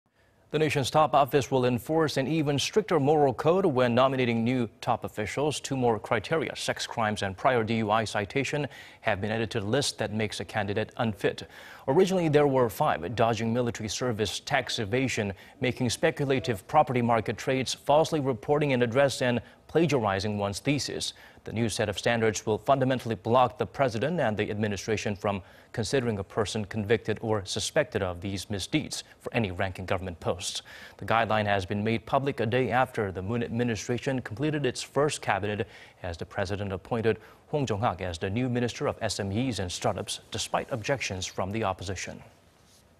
Blue House to enforce stricter rules when picking top governmental and public officials
5대비리에서 7대 비리로... 성 비위, 음주운전도 고위공직 원천배제 기준에 포함

  The nation's top office will enforce an even stricter moral code when nominating new top officials.
  Two more criteria, sex crimes and prior DUI citation, have been added to the list that makes a candidate unfit.
Originally there were five: dodging military service, tax evasion, making speculative property-market trades, falsely reporting an address, and plagiarizing
one's thesis.
The new set of standards will fundamentally block the president and the administration from considering a person convicted or suspected of those misdeeds, for any ranking government posts.
The guideline has been made public a day after the Moon administration completed its first cabinet,... as the president appointed  Hong Jong-haak... as the new minister of SMEs and startups, despite objections from the opposition.